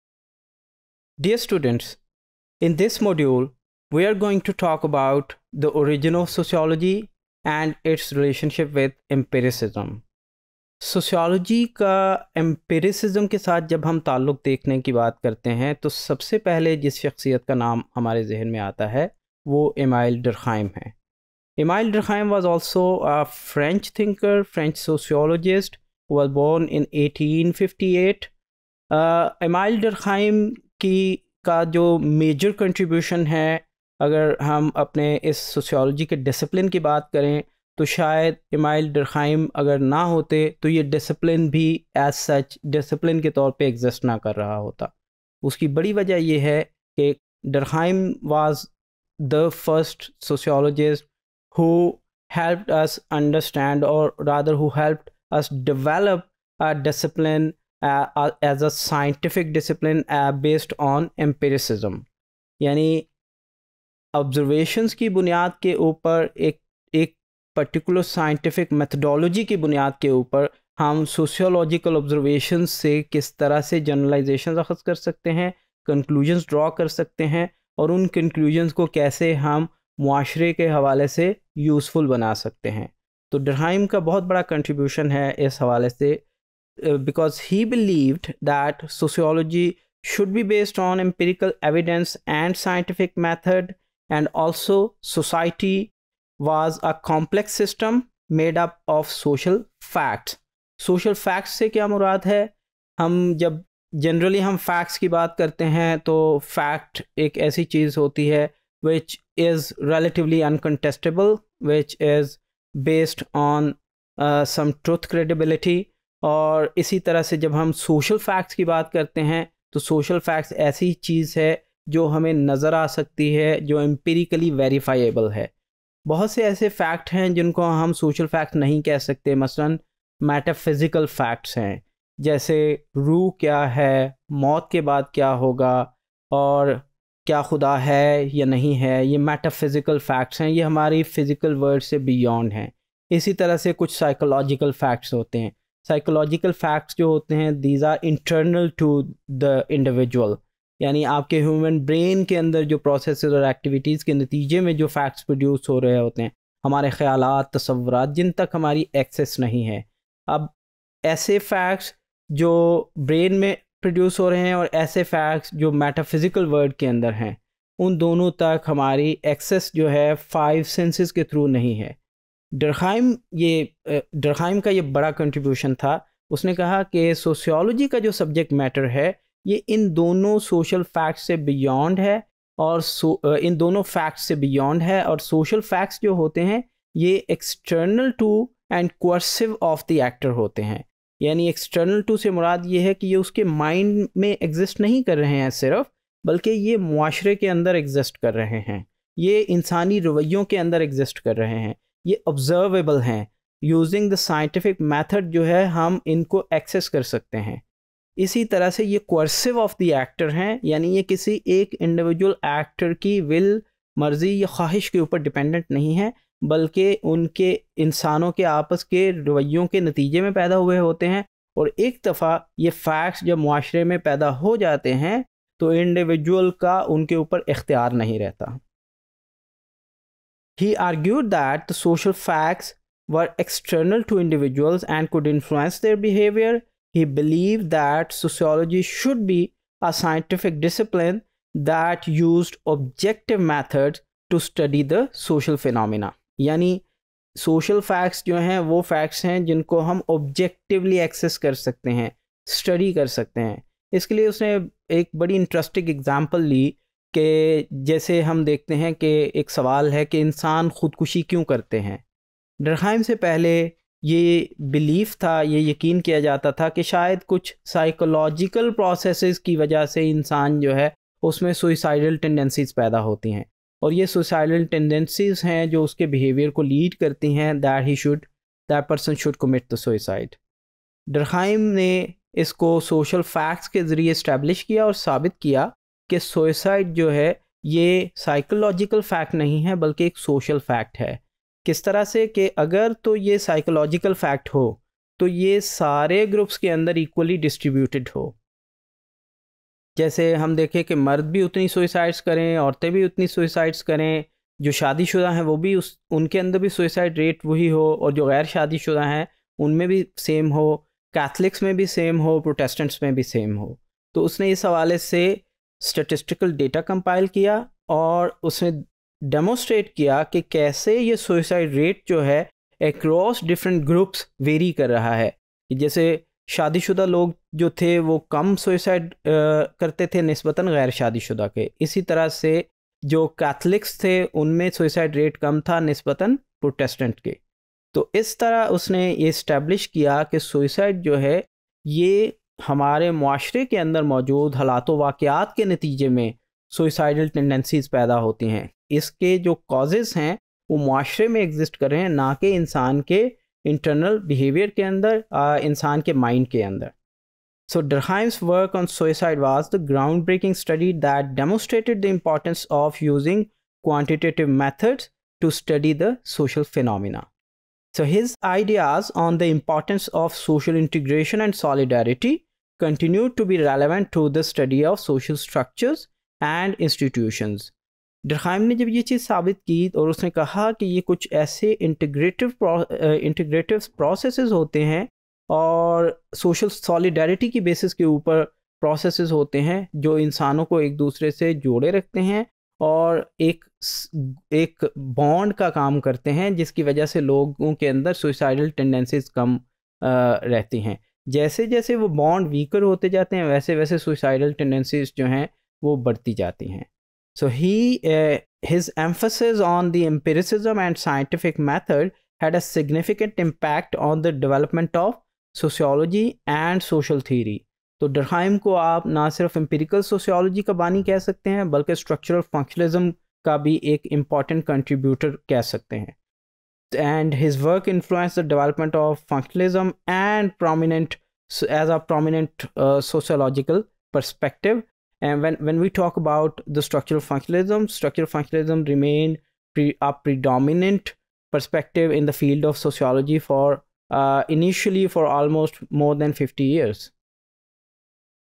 Origin of Sociology Empiricism Emile Durkheim 1858-1917 | Introduction to Sociology| SOC101_Topic006
Topic006: Origin of Sociology - Empiricism (Emile Durkheim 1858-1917),
By Dr. Imran Sabir